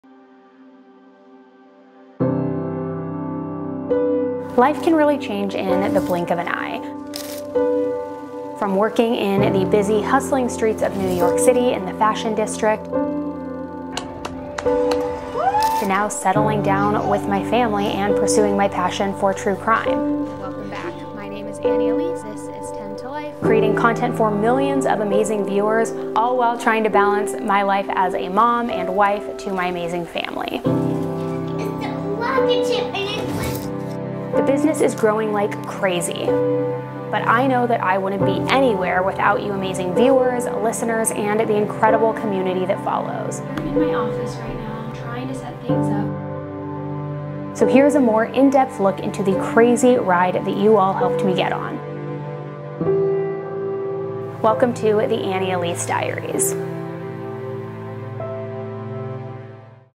Welcome to The Annie Elise Diaries!
Hey everyone! 🤗 I am so excited to share my newest project with you:
The Annie Elise Diaries
I will be giving you a peek behind the curtain into my everyday life, including the ups and downs, as well as behind-the-scenes looks at my true crime projects, fashion, lifestyle, and travel. I want you to be a part of it all, so let's go on this adventure together!

If you're here for the first time, make sure to check out my true crime channel